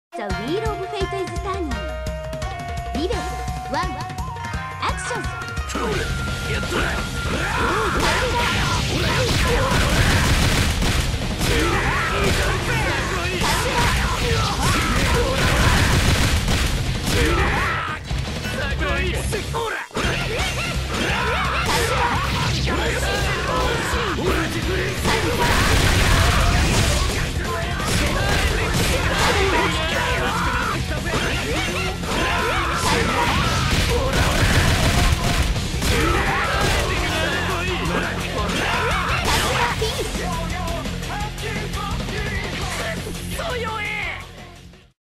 Online Flilth
Its because of shit like this that terumi players continue to have a bad rep. 100% of the terumi players i have fought on steam do this. there is a difference between trolling skillfully, and just being a dick. Go kill yourselves pls.

and yes, the thumbnail is godly with that leg angle.